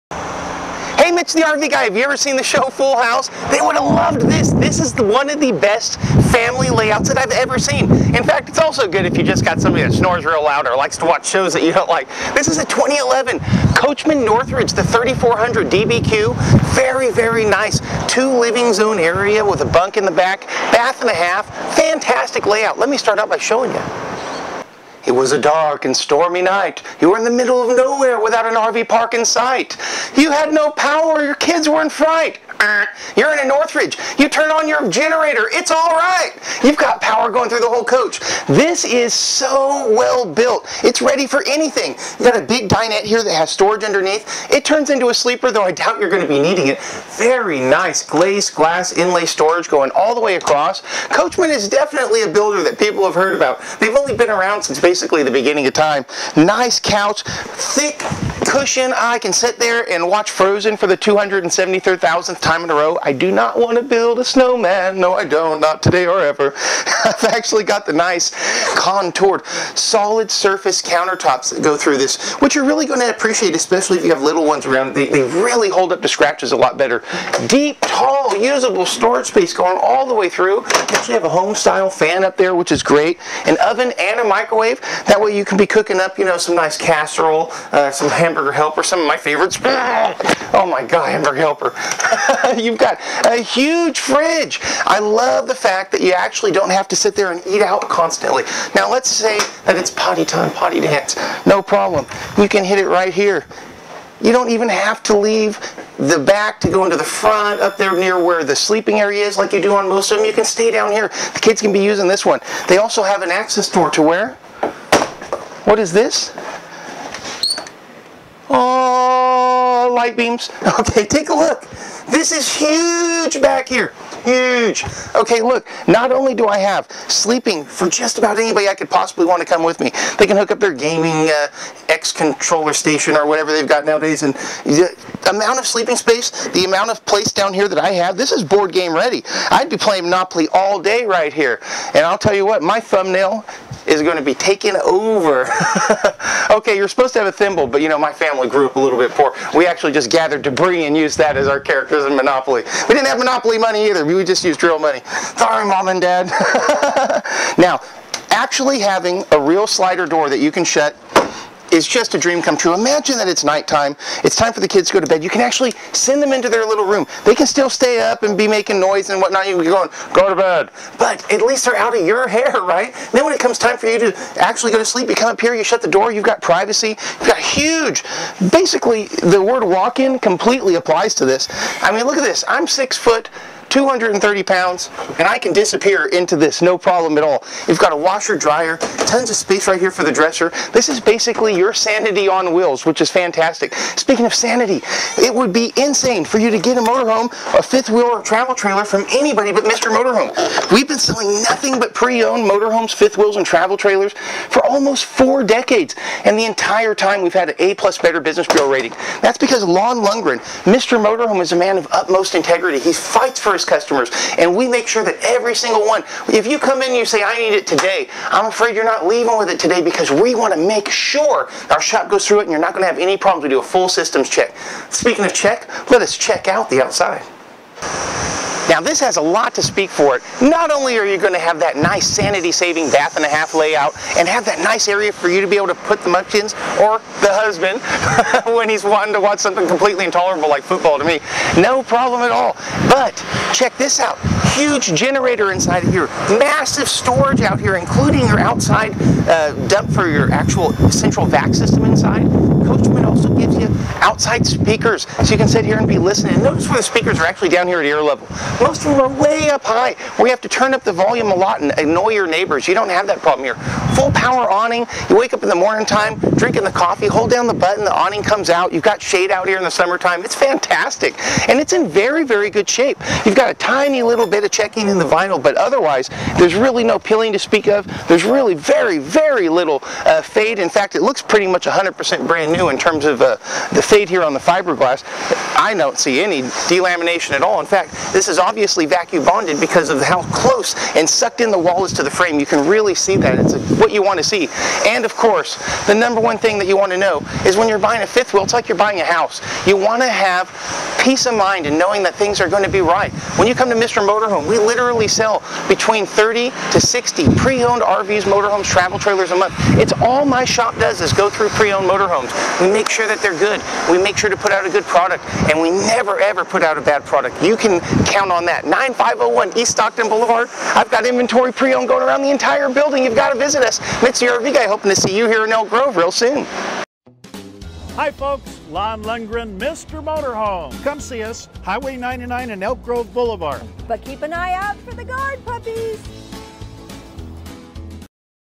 Mr Motorhome 2011 Coachmen North Ridge 340 DBQ preowned fifth wheel for sale 1150A Sacramento Ca
Visit us at 9501 E Stockton Blvd Elk Grove Ca 95624 or online at MrMotorhome.com